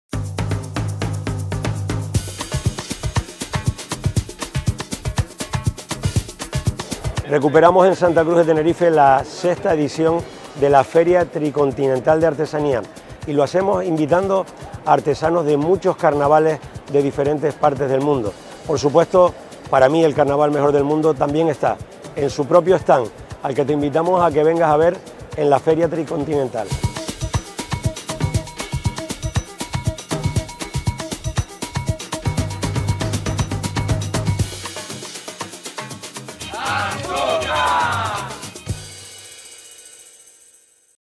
🎭 El Carnaval de Santa Cruz, presente en la Feria Tricontinental 💙💚
🔝 Las carnestolendas chicharreras se muestran en un gran stand de 352 metros cuadrados en el Recinto Ferial de Tenerife donde se desarrollará un diverso programa de actividades culturales y de ocio.